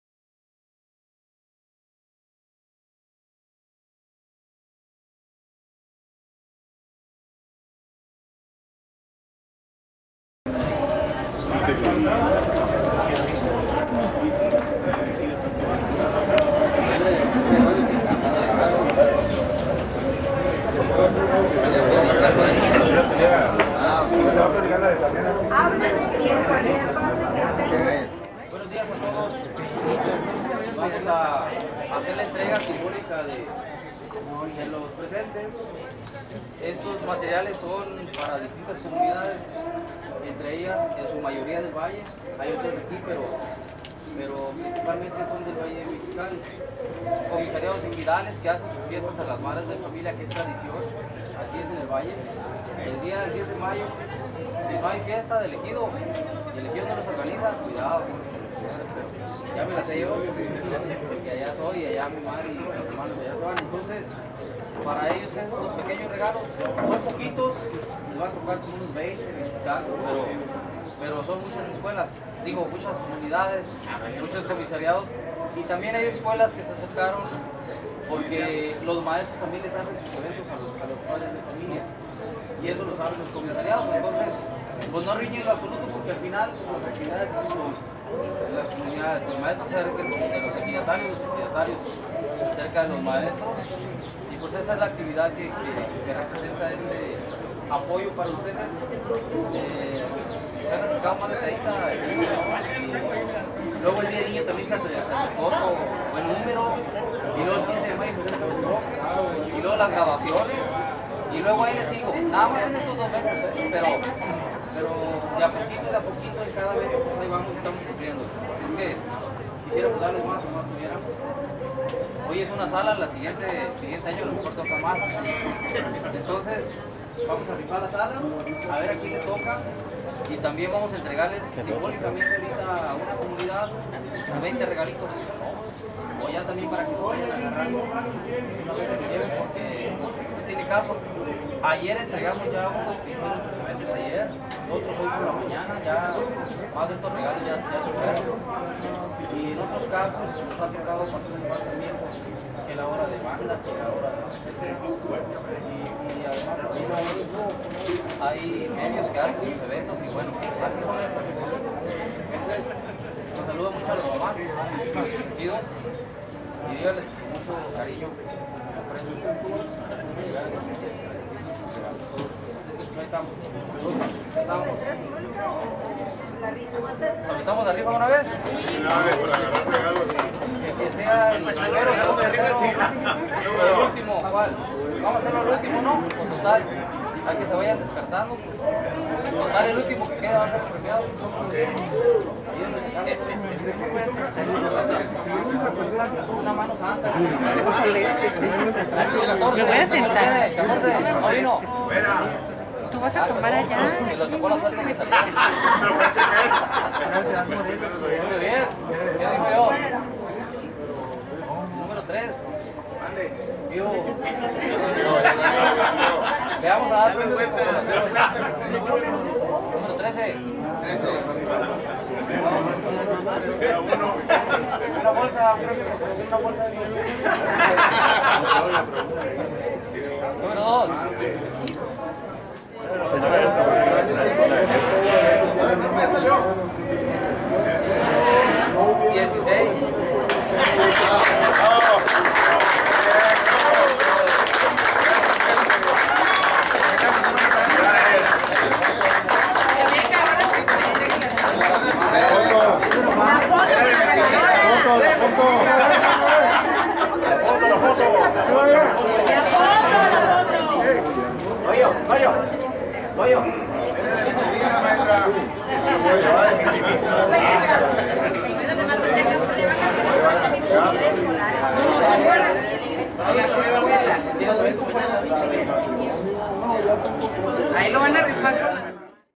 Dip. Gregorio Carranza Hernández, en uno de sus eventos entrega regalos
Continua el trabajo  de gestión social para la ciudadanía